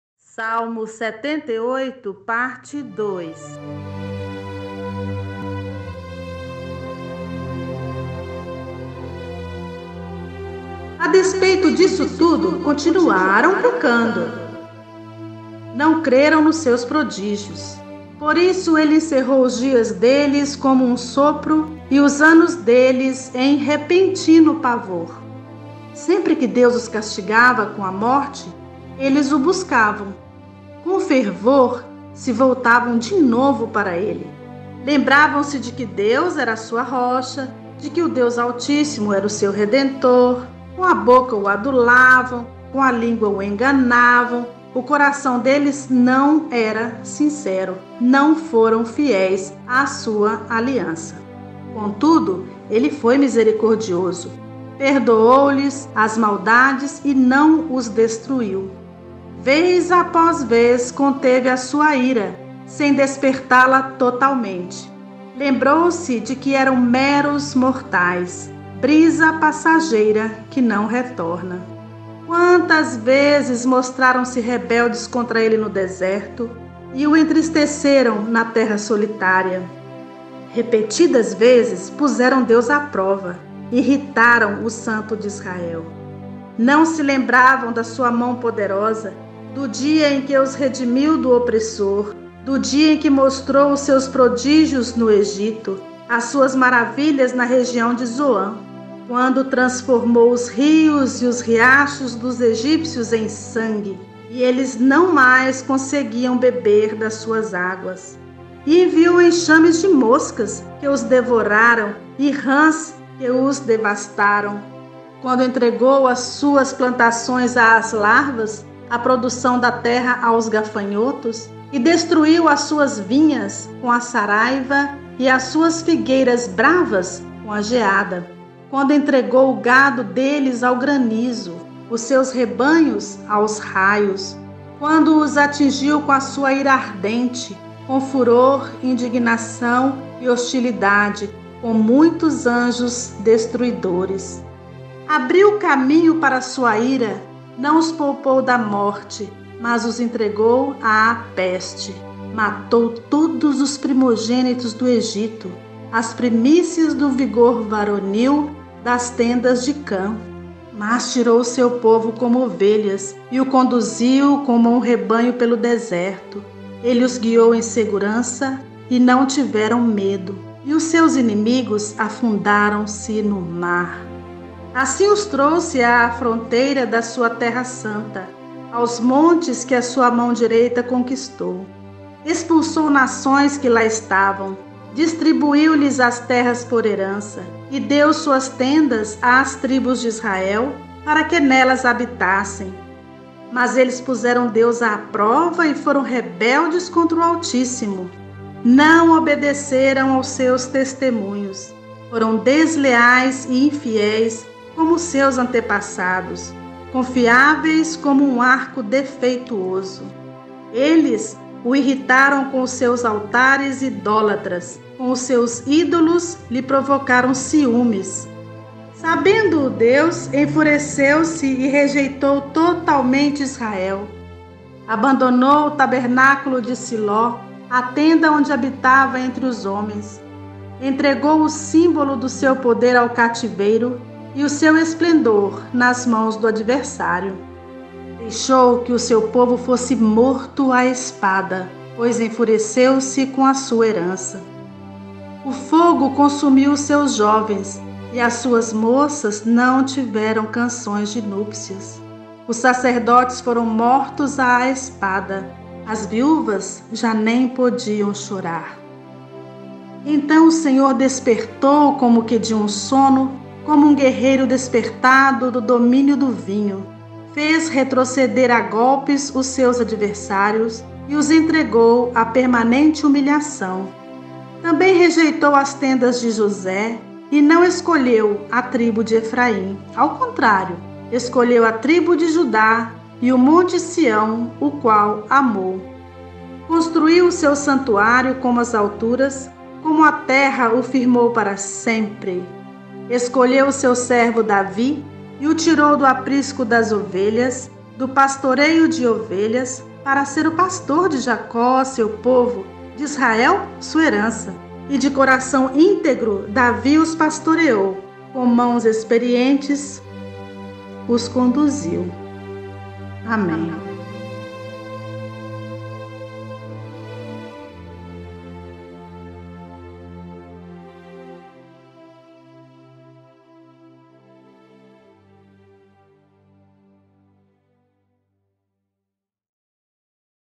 LEITURA DIÁRIA - Salmos 78 - PARTE 2
Olá meus amigos que nos acompanham aqui no canal LouvandoSempre. Em parceria com Moema Leite apresentamos o Salmo 78 - PARTE 2. Nos acompanhem...